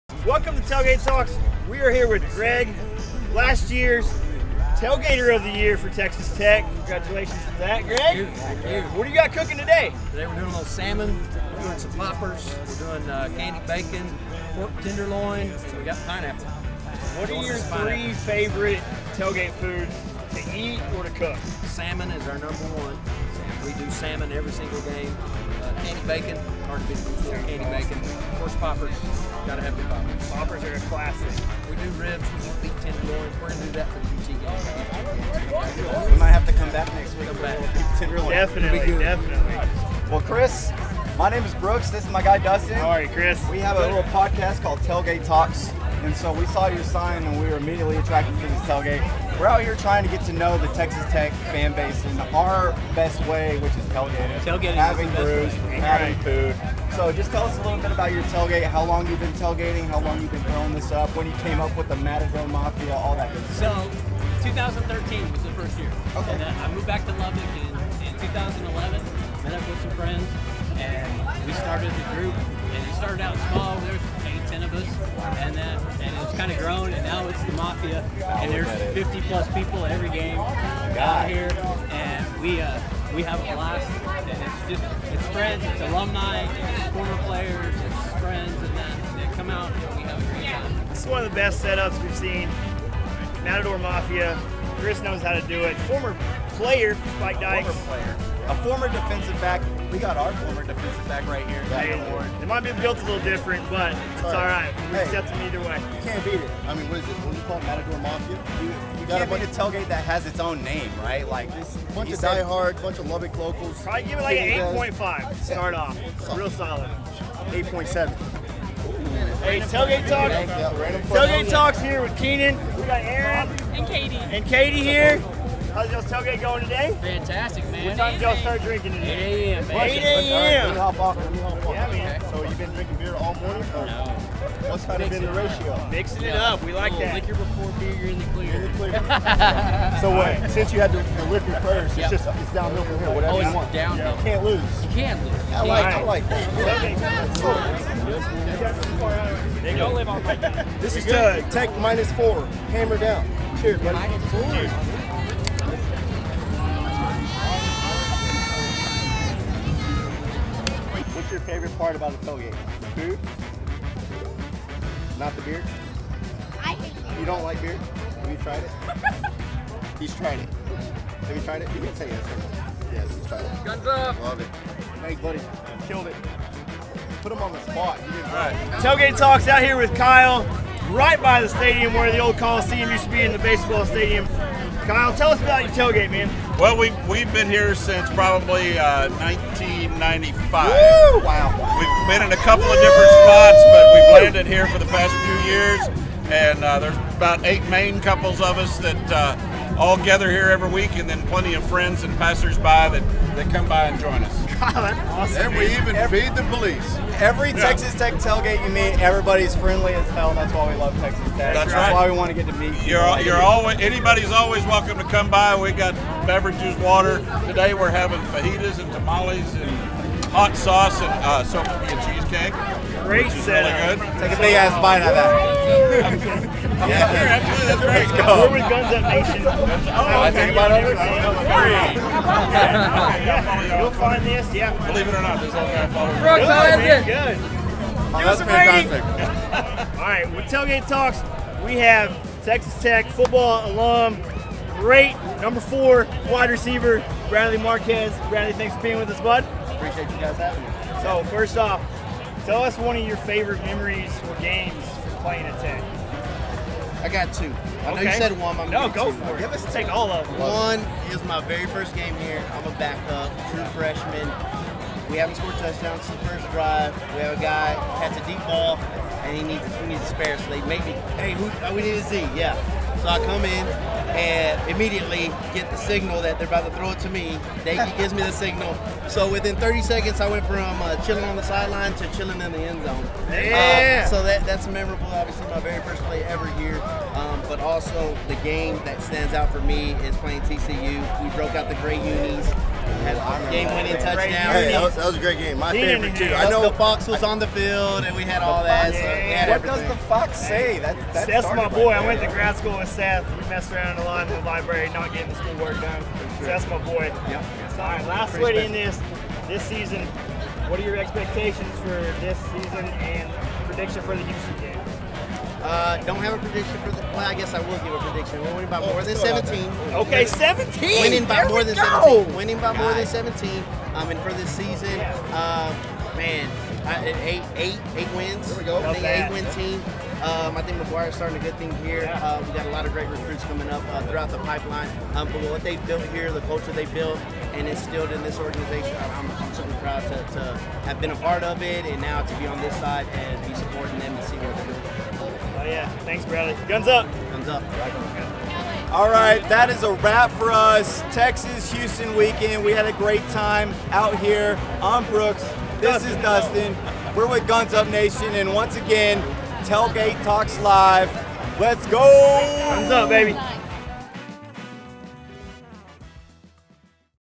Houston vs Texas Tech Tailgate Crashers!
It is never too late for our Tailgate Crasher Highlights from the UH game. We were out crashing more tailgates before the exiting double overtime victory over the Houston Cougars. Had an absolute blast getting to meet Red Raider fans and tailgating with people like the Matador Mafia. Great time as always before a great win.